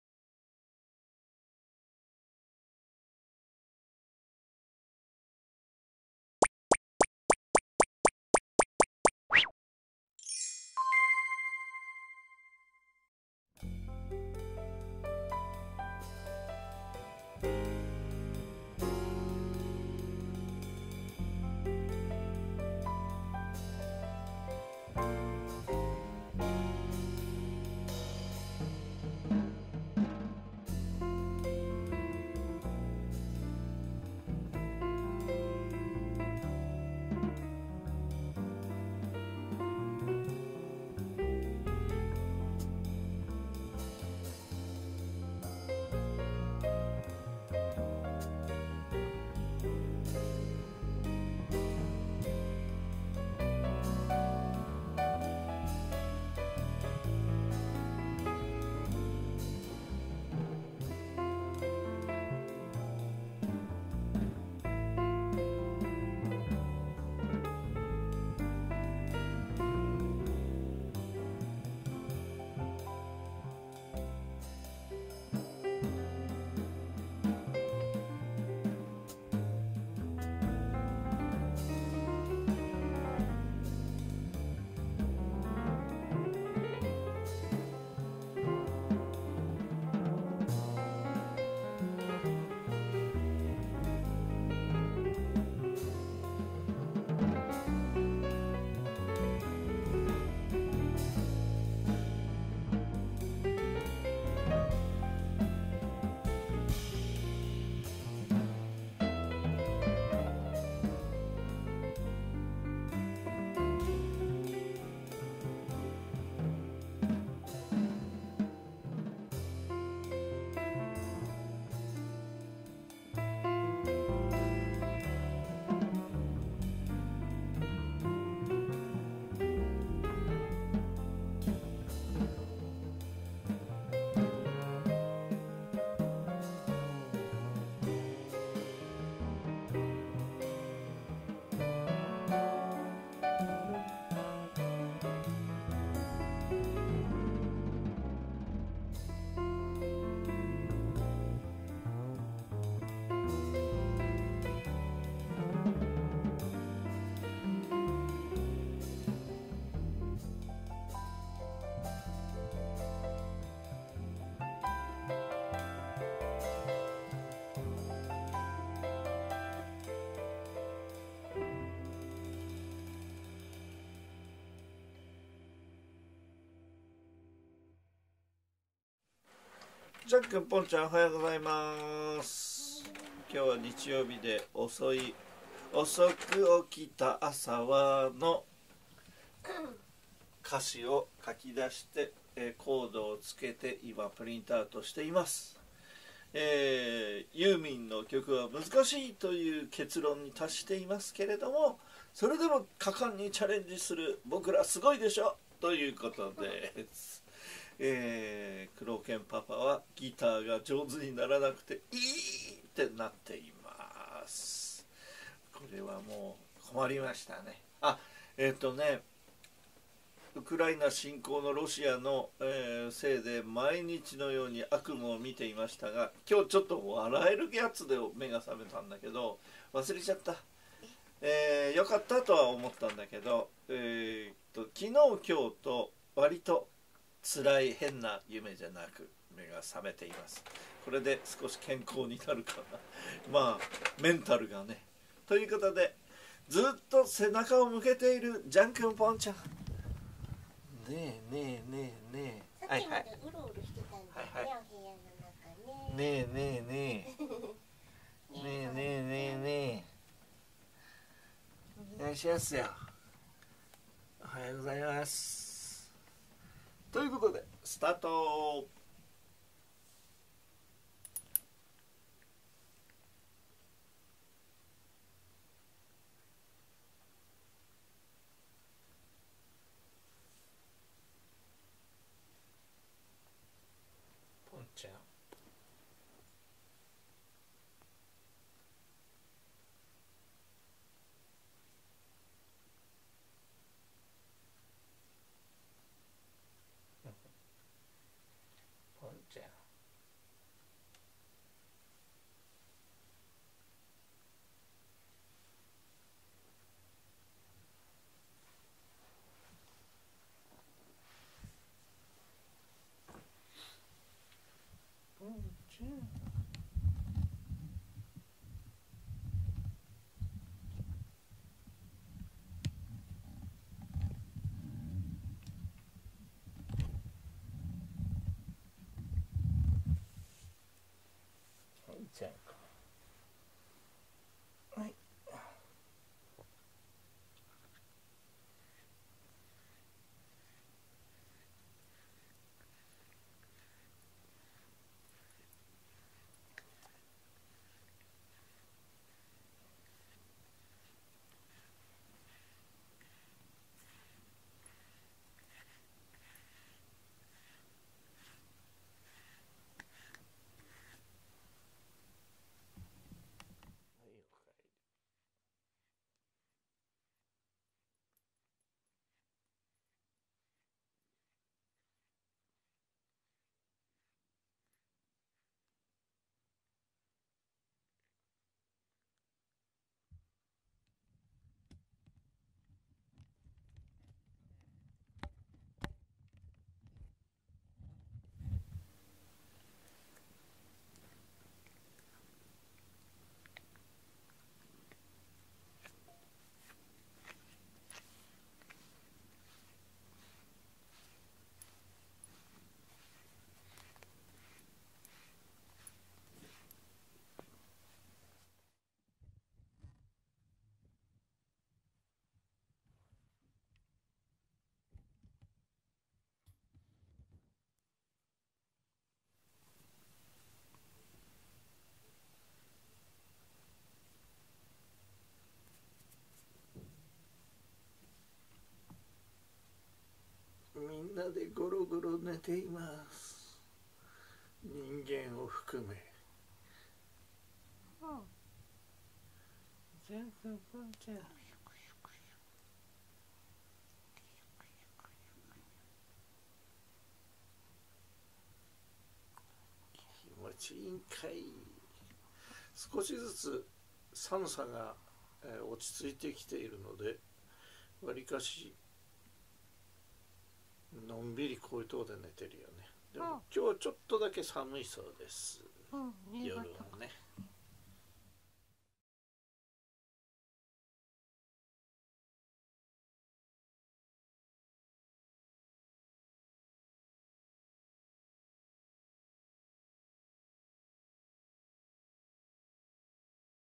春が近くなって暖かいのでみんなでお昼寝の日曜日【Jean & Pont 2779】2022/3/6 保護猫ジャンけんポン
世界は最悪の状態で毎朝悪夢で目覚める日が続きましたが、季節は関係なく暖かい春に向かっています。
日曜日の午後に夫婦と２ニャンはうとうと眠りにつき半日を無駄に過ごしてしまいました。
今苦しんでいる人たちに申し訳ない、何もできない僕らを悔みます。
世界が憎しみで包まれない様に暖かい日差しで寛げる様に祈ります。